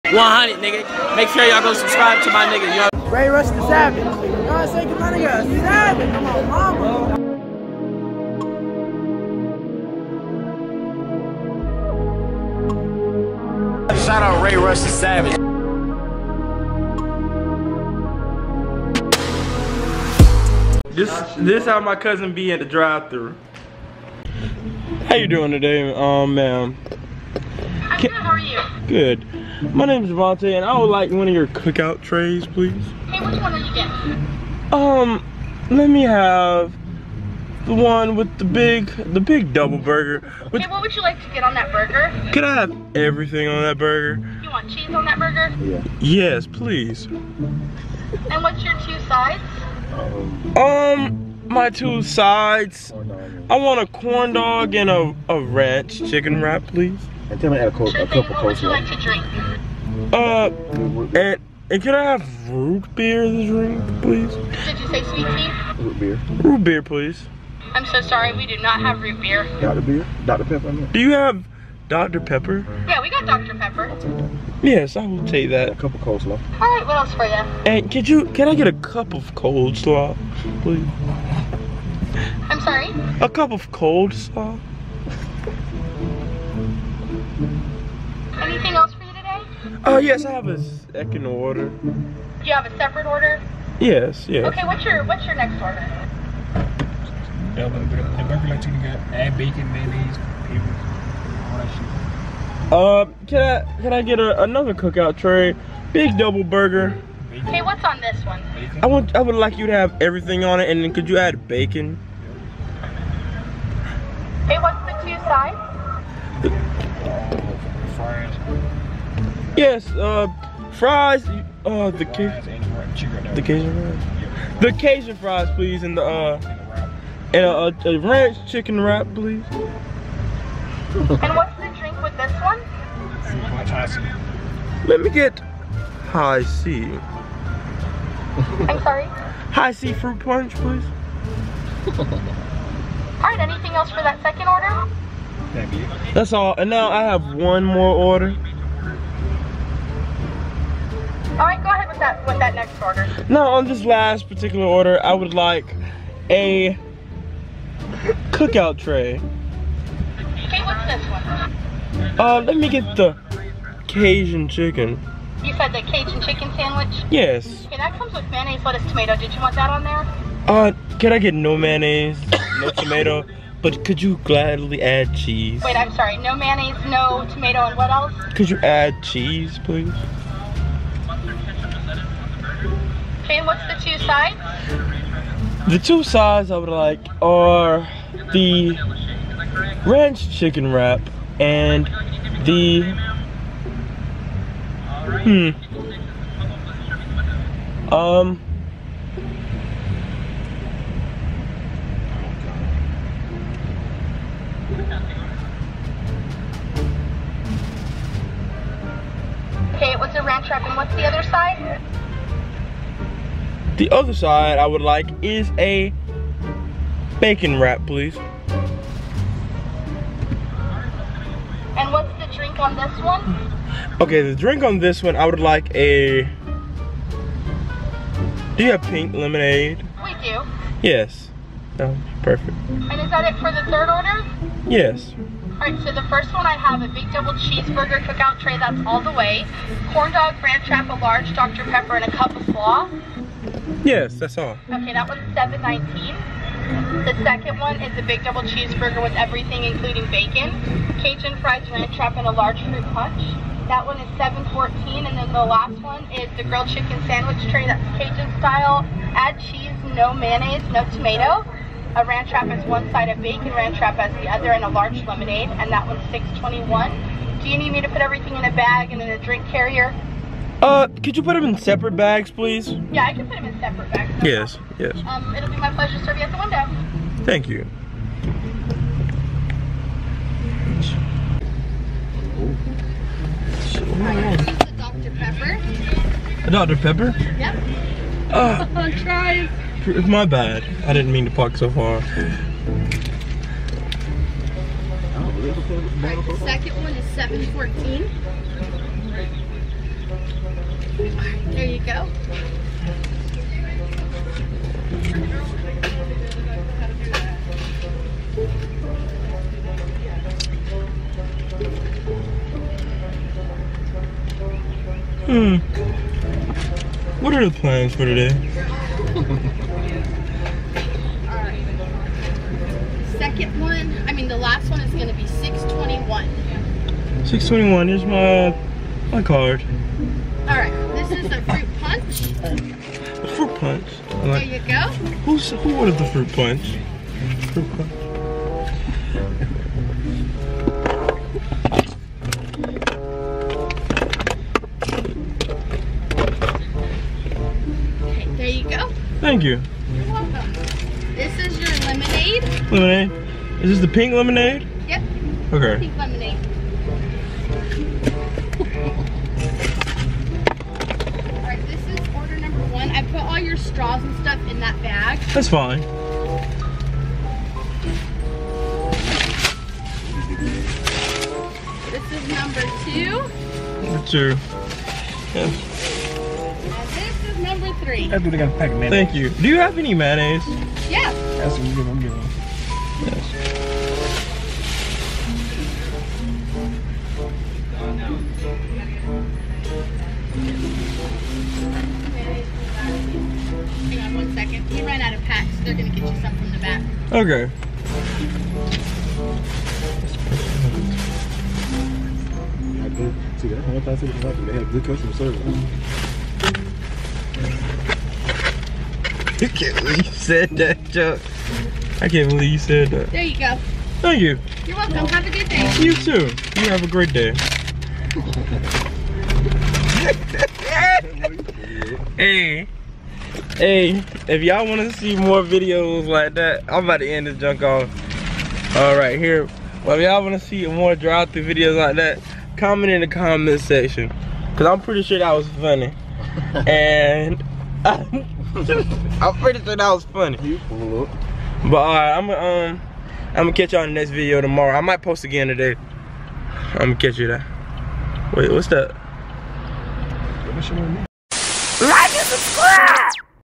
HILARIOUS COOK OUT DRIVE THRU PRANK!!! | Voice Change
Had Them Confused...